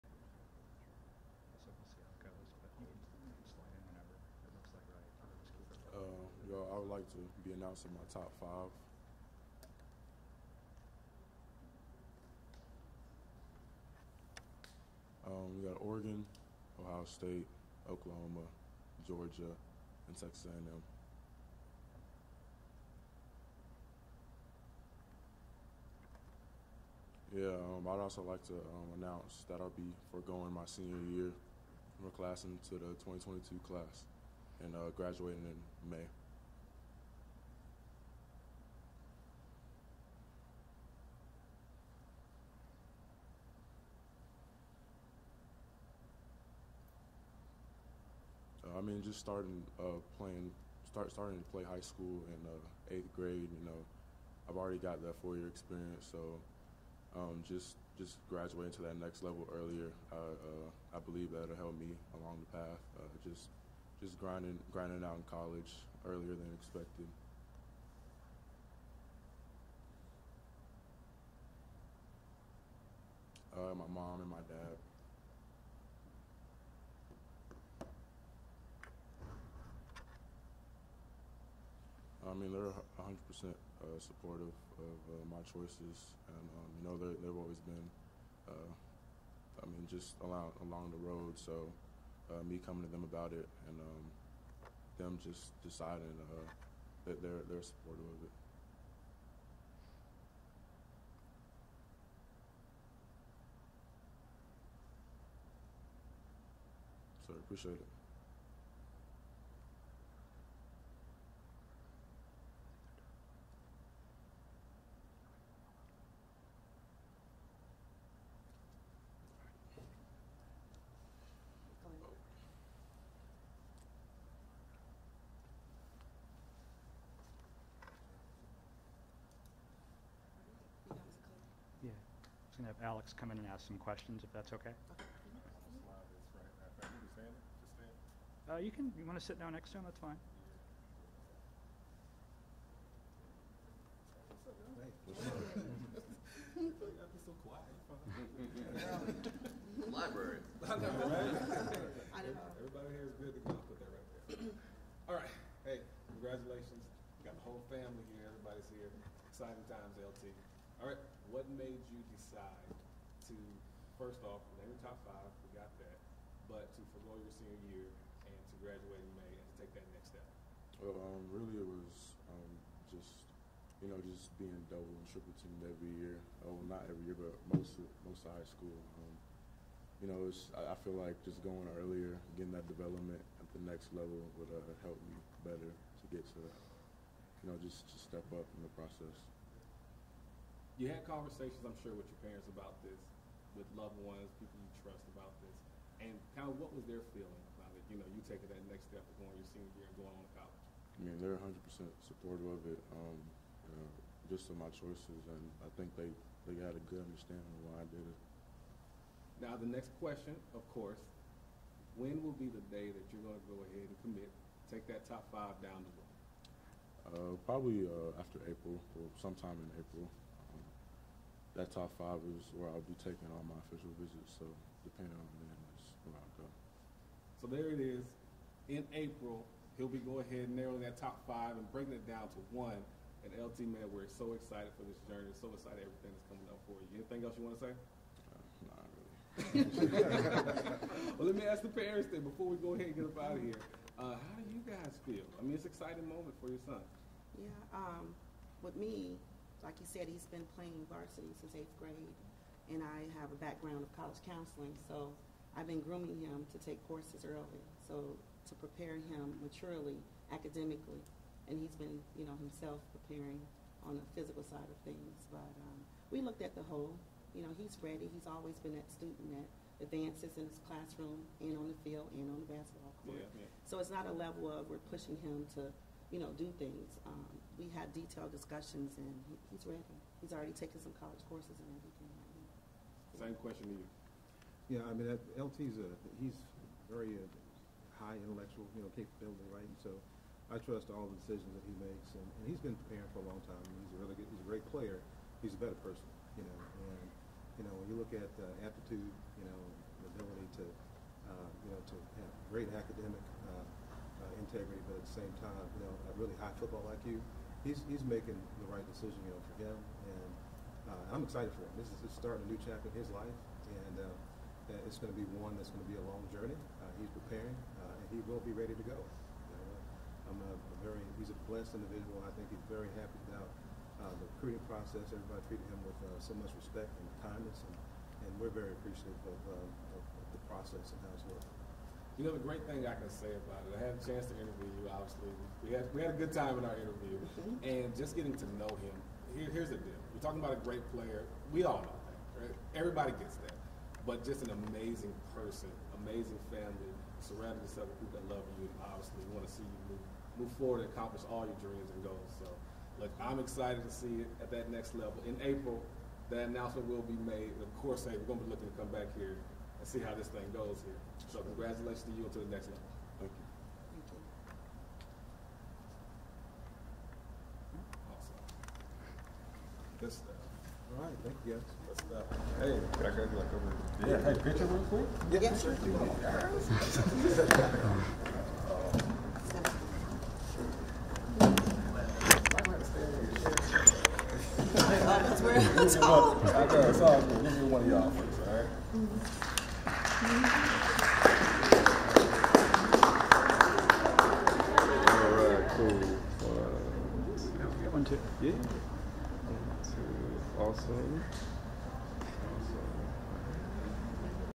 Milton's LT Overton to forgo senior season, reveals top 5 college choices
The Milton High School football player made the announcement on 11Alive on Thursday morning.